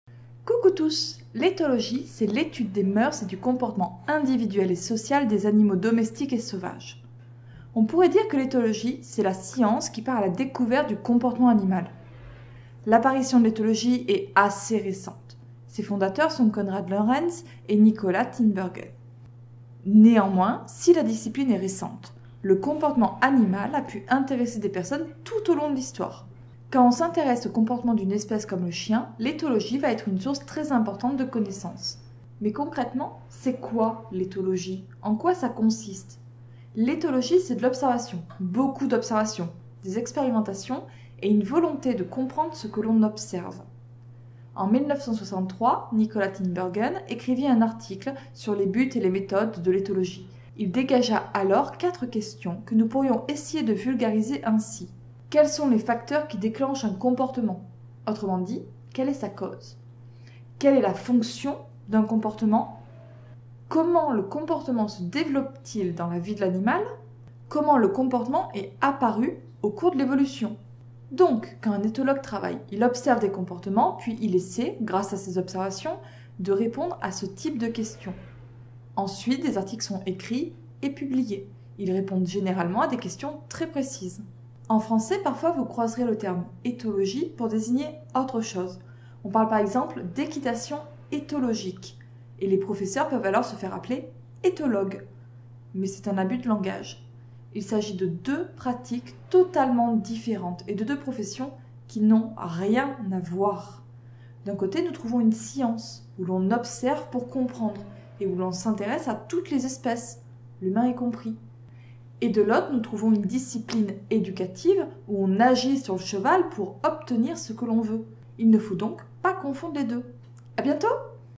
C'est quoi l'éthologie ?
Aujourd'hui, nous parlons rapidement de l'éthologie dans une petite présentation. Je tiens à remercier Kévin pour sa relecture, ses corrections et son aide précieuse !